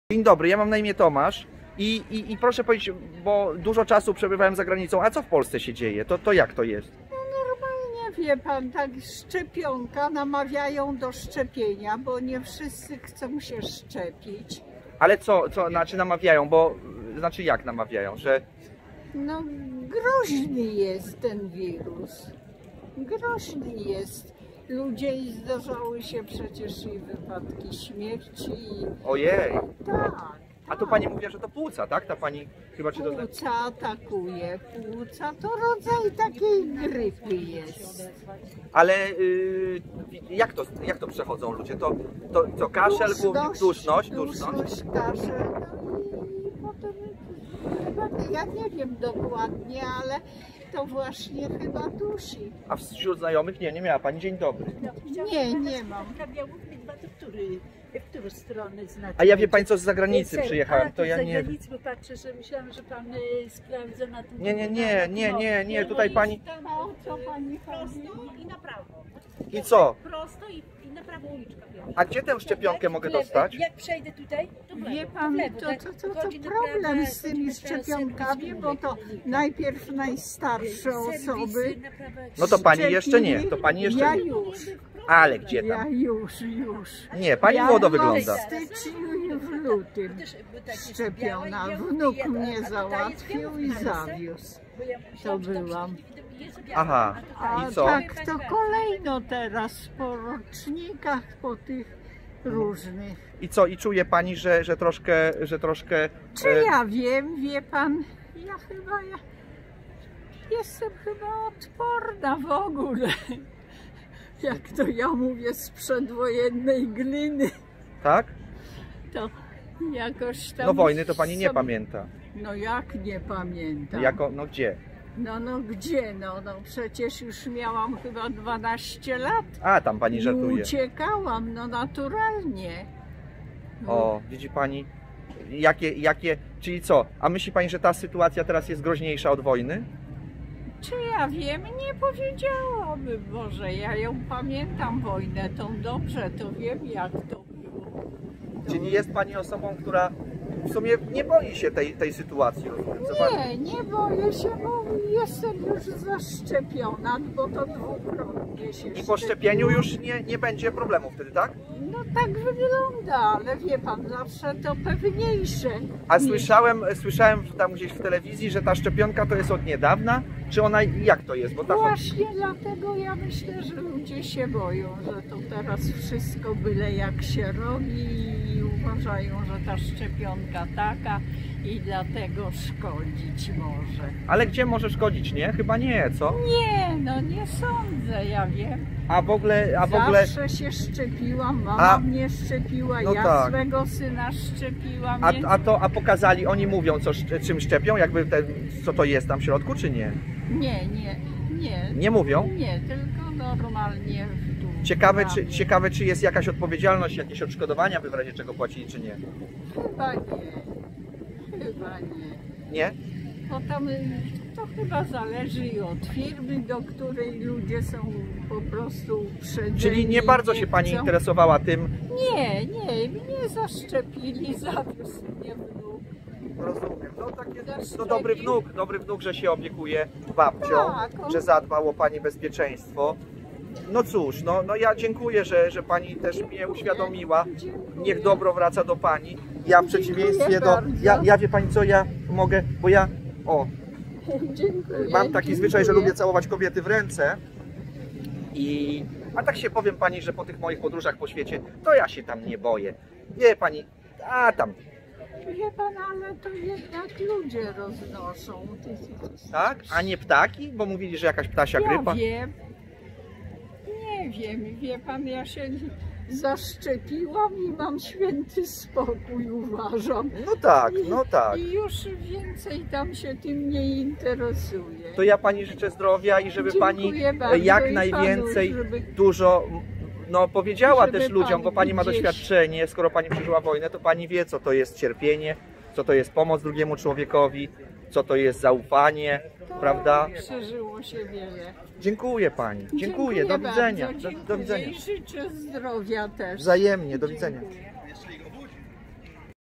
"Święty Spokój"
Wywiad z kulturalną Panią - która przeżyła druga wojnę światową - smutna i bolesna prawda o kolejnej. Tym razem totalnie światowej, globalnej...